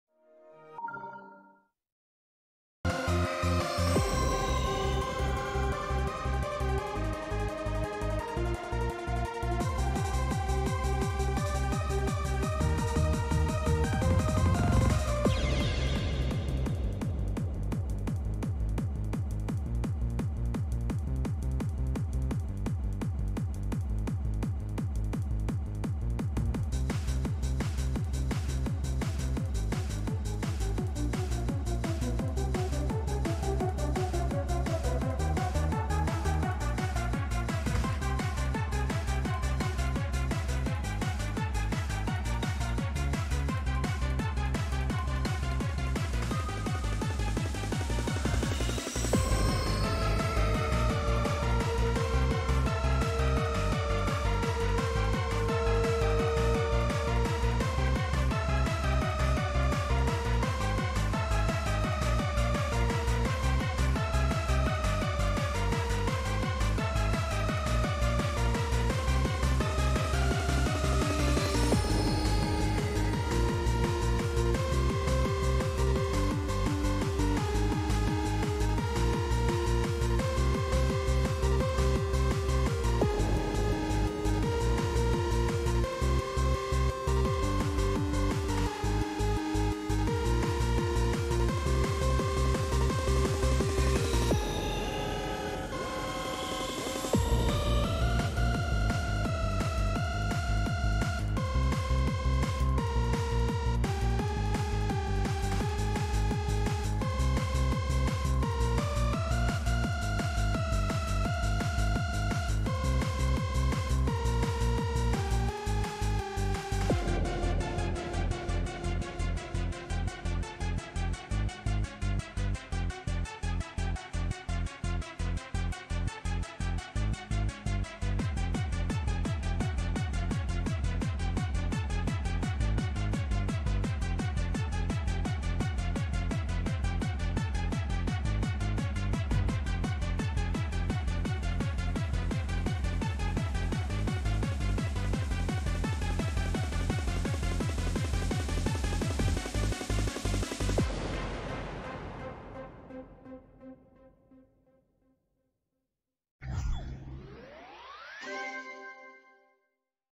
[Showcase] “El Impecible” // Full Layout // By: GoesByJ and Duque01 // The Flawless Remake
This layout was made by 2 amazing people, Duque01 and GoesByJ, they made the layout quite a bit ago but never got people to decorate, so I got in contact with Duque and asked if I could take the layout off his hands and his was happy to, so please go check out their channels below, and this was also recorded by GoesByJ and I will need some decorators, so do join my Collab server down below or the pinned comment in the comment section.